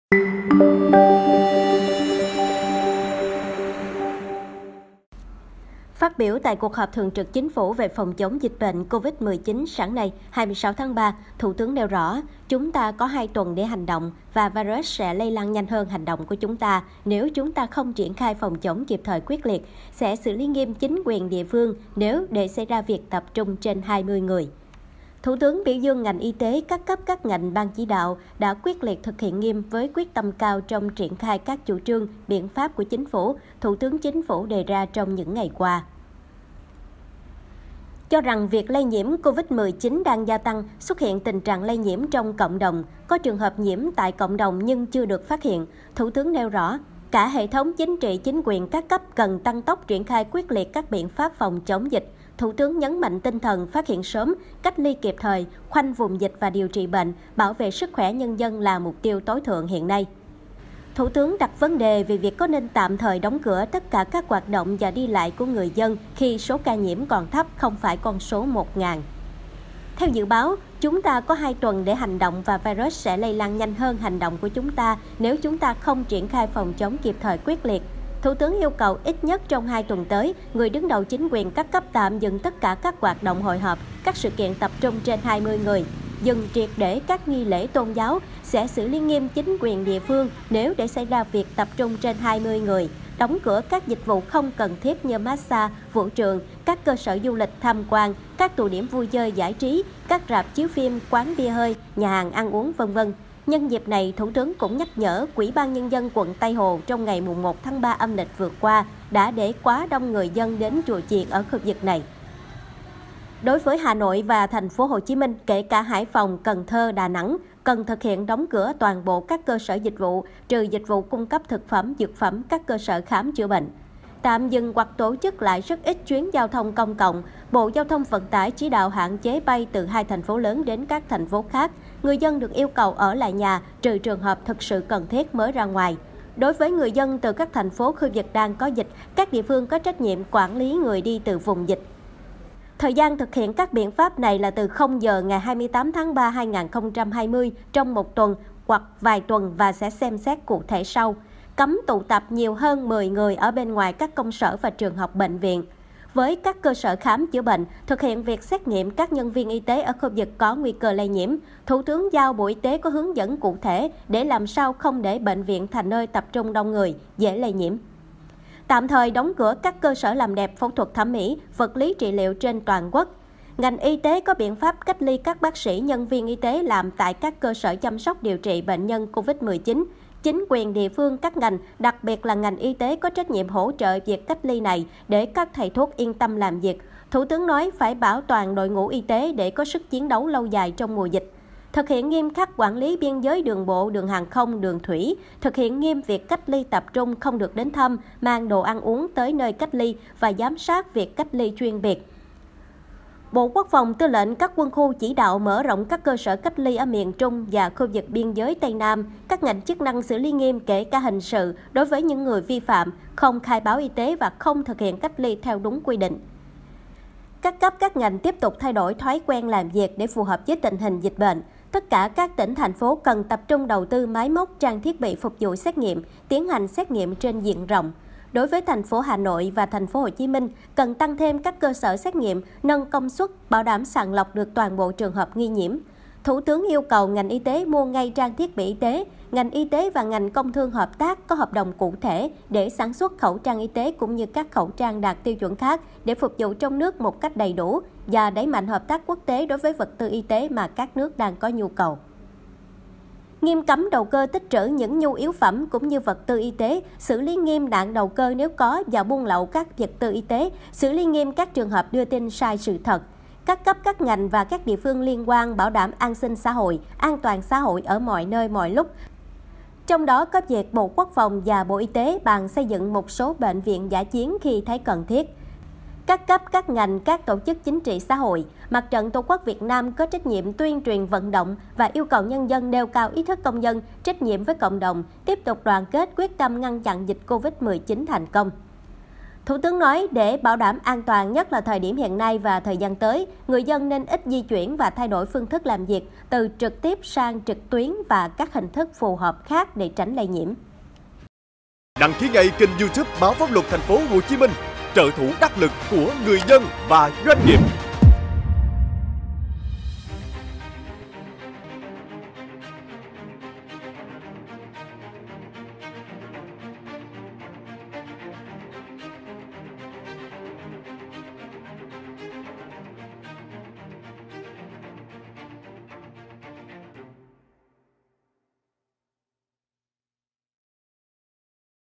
Để tụ tập trên 20 người, lãnh đạo địa phương sẽ bị xử lý nghiêm - PLO
Phát biểu tại cuộc họp Thường trực Chính phủ về phòng chống dịch bệnh COVID-19 sáng nay, 26/3, Thủ tướng nêu rõ: Chúng ta có 2 tuần để hành động và virus sẽ lây lan nhanh hơn hành động của chúng ta nếu chúng ta không triển khai phòng chống kịp thời, quyết liệt. Sẽ xử lý nghiêm chính quyền địa phương nếu để xảy ra việc tập trung trên 20 người.